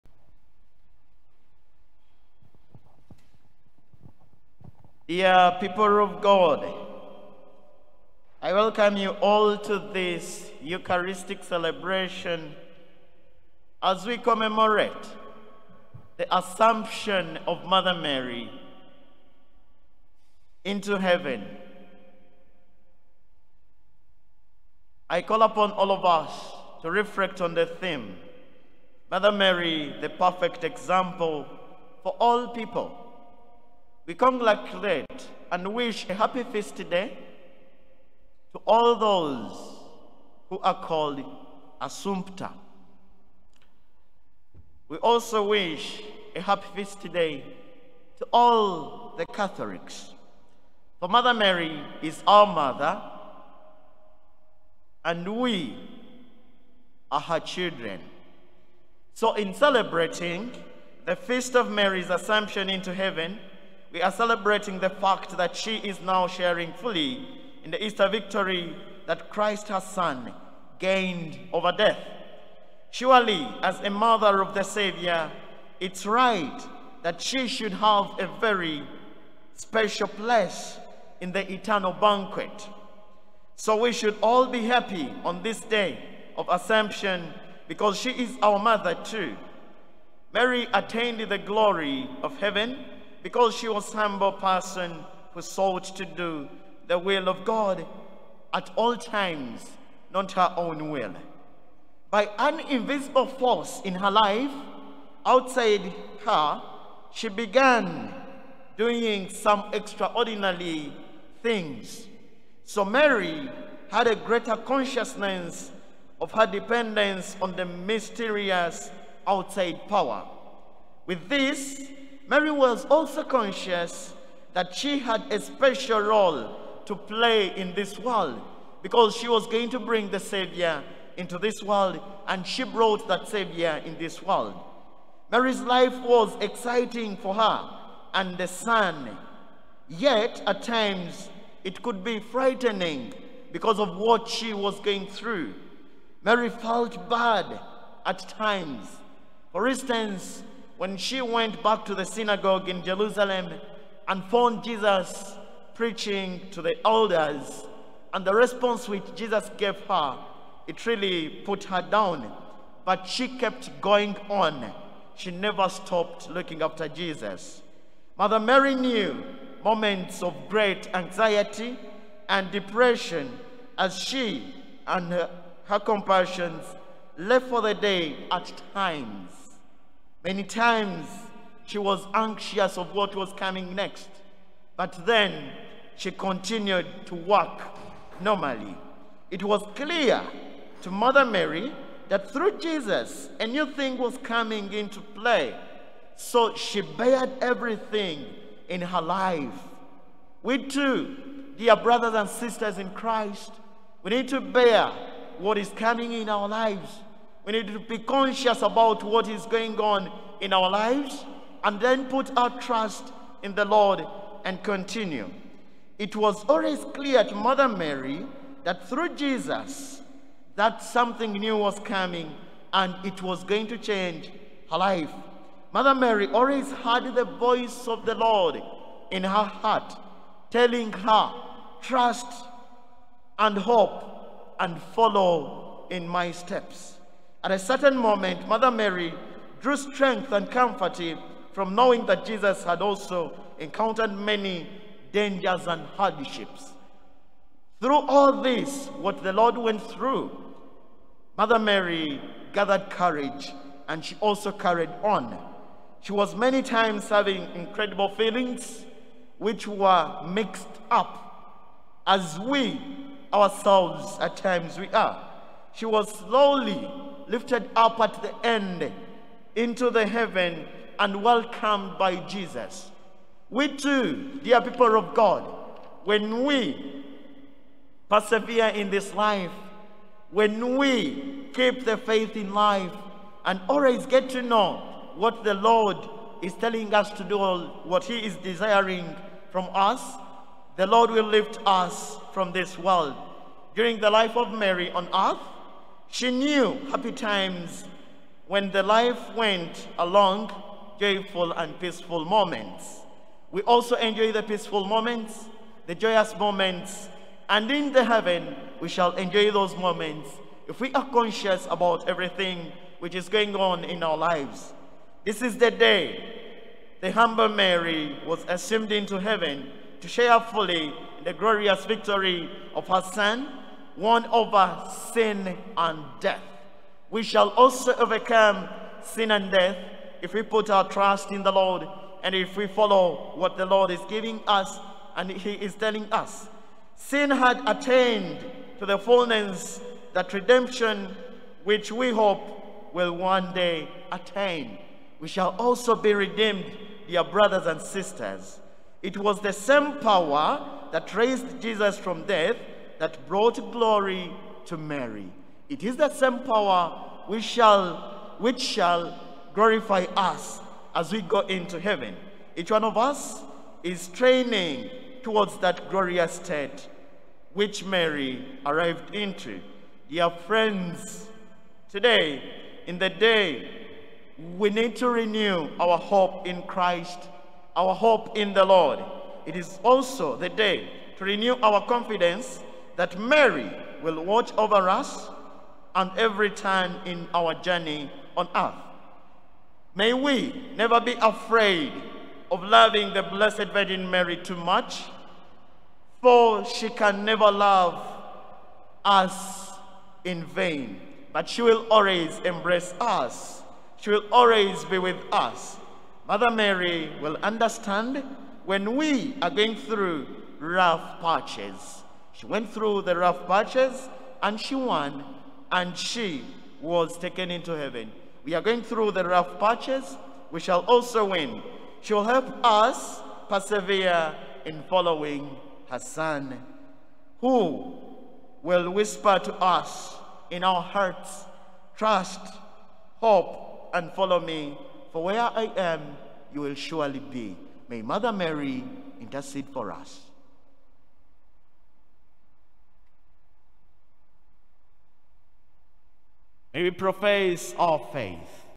Assumption Homily By Fr Alex Kambagira, sunday 15th August 2021
Assumption Homily 15th August 2021 By fr Alex From Lubaga Cathedral Parish